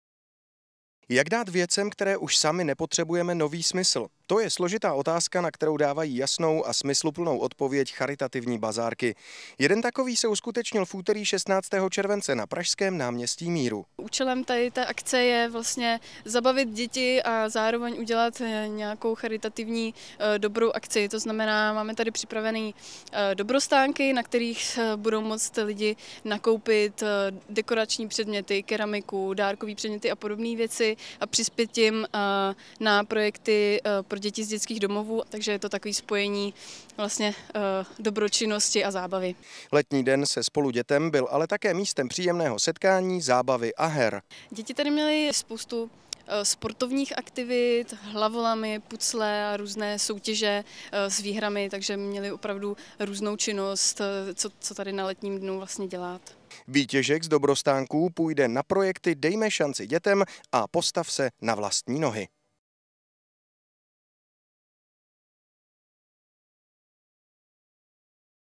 Letní den se Spolu dětem
Den plný her, soutěží, zábavného programu a charity pořádala nezisková organizace Spolu dětem 16. července 2013 na Náměstí Míru v Praze. Na malé a velké návštěvníky čekaly zábavné hry, zdobení sušenek, Dobrostánky Spolu dětem a posezení u kávy dobré palačinky. Výtěžek z dobrostánků ve výši 33 413 Kč půjde na vzdělání dětí v dětských domovech na projekty Dejme šanci dětem a Postav se na vlastní nohy. Děkujeme za vaši návštěvu! :)